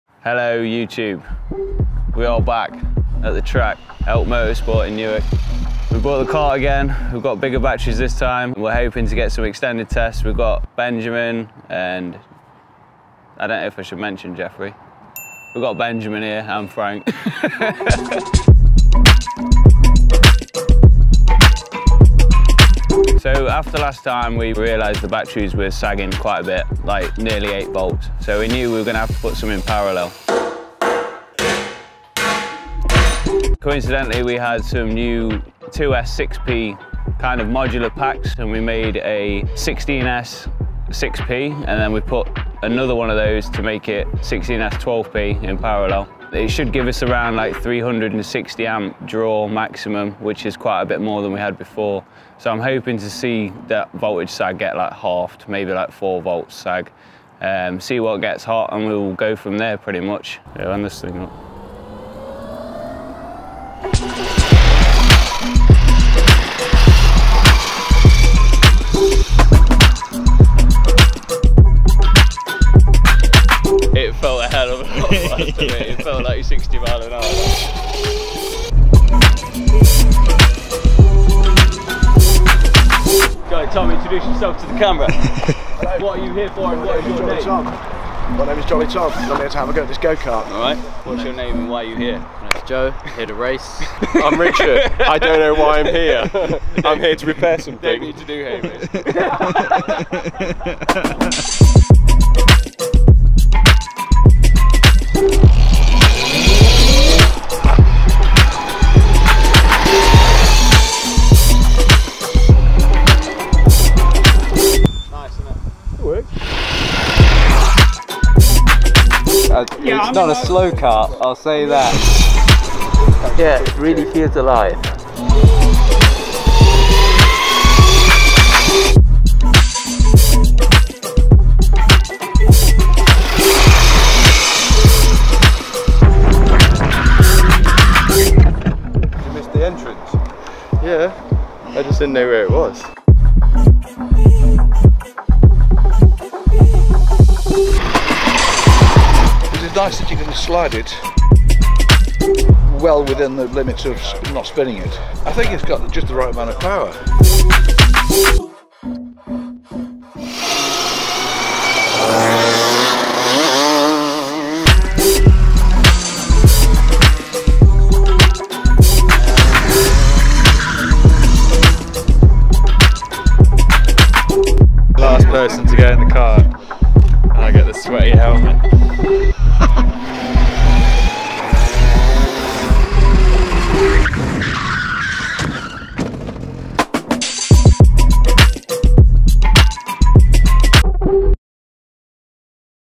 Trampa Electric Go-Kart | Crew Test Drive
After the 1st track day we made some updates to the kart.  We then went back to give it another go.